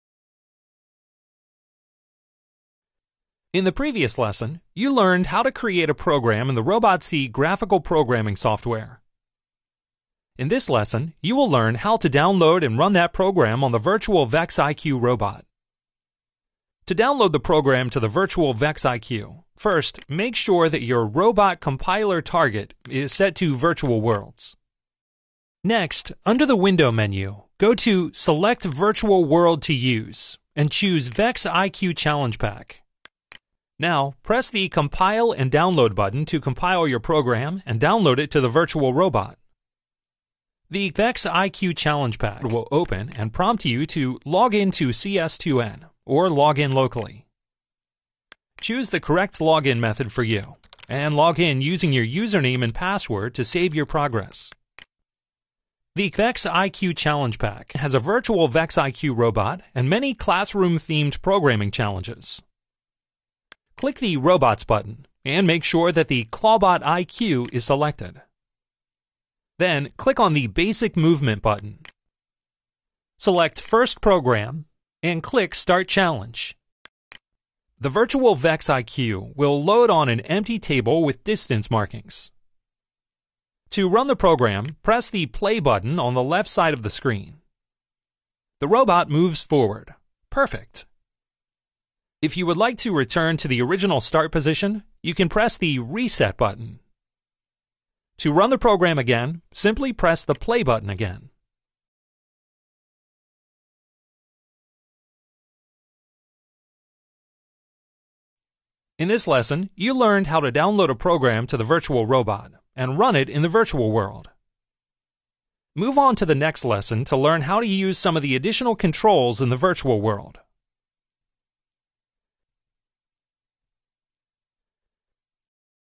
Part 2: Downloading your Program to the Virtual VEX IQ
This video shows how to download an run a program on the Virtual VEX IQ in the Challenge Pack for VEX IQ Robot Virtual World. This video is part of the Introduction to Programming VEX IQ Curriculum